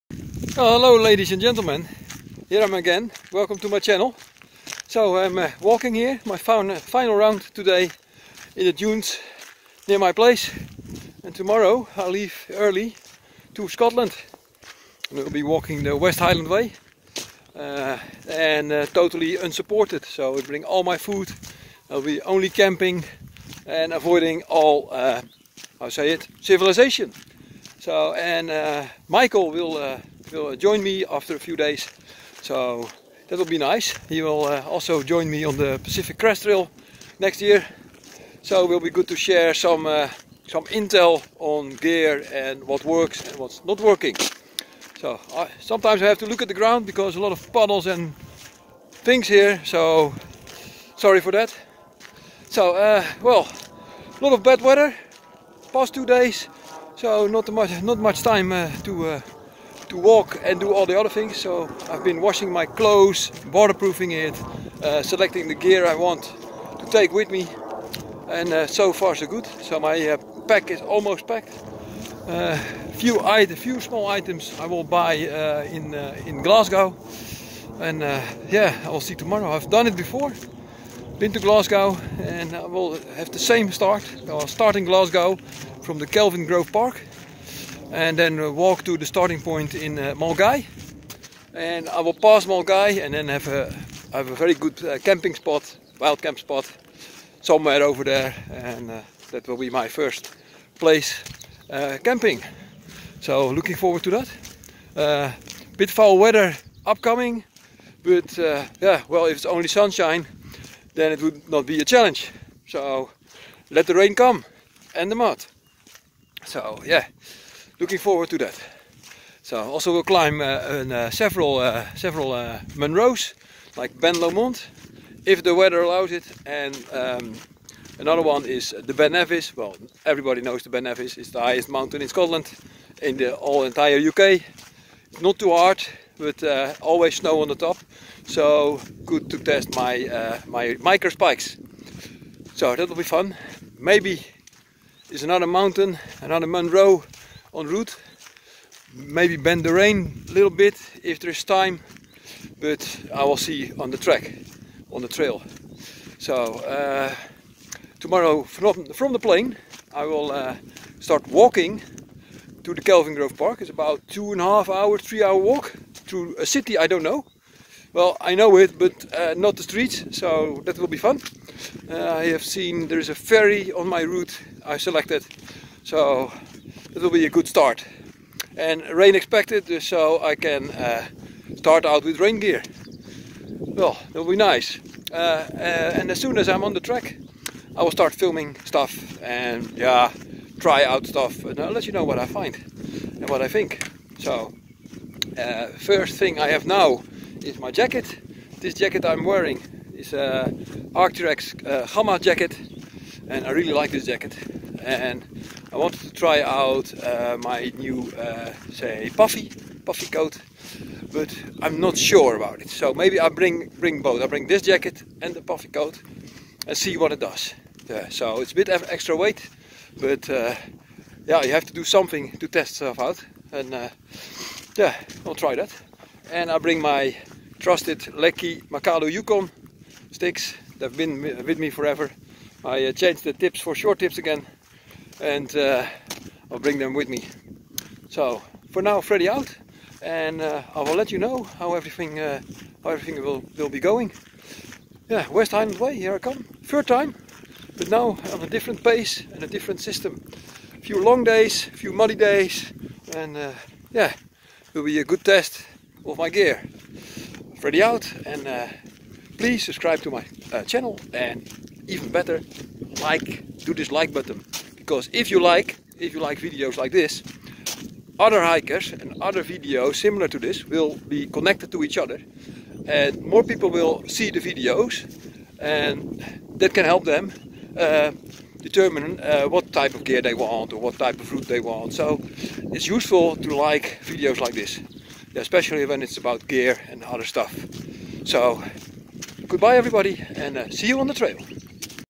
Pacific Crest Trail - CLASS 2019 - Prep day 0 West Highland Way
Some talk about my upcoming backpack trip